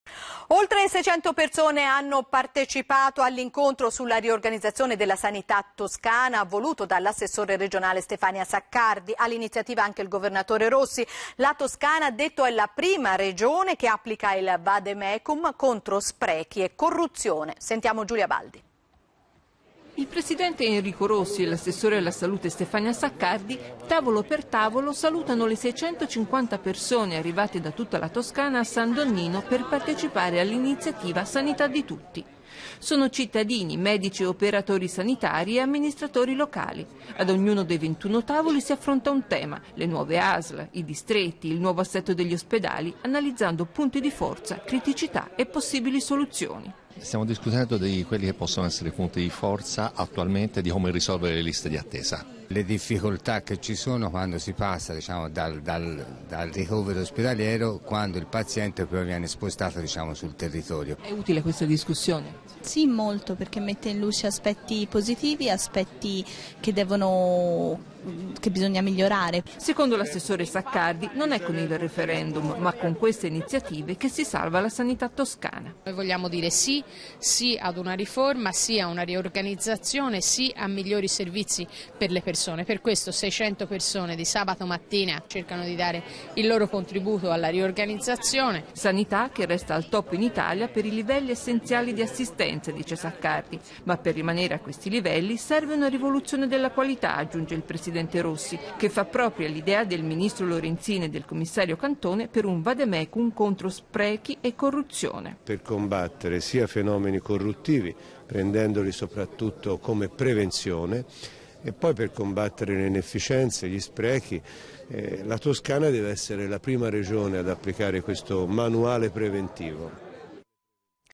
Sabato 7 a San Donnino 650 cittadini toscani a parlare di sanità
650 persone ai tavoli per discutere di sanità e di come riorganizzarla. Presenti Enrico Rossi e Stefania Saccardi
Guarda il servizio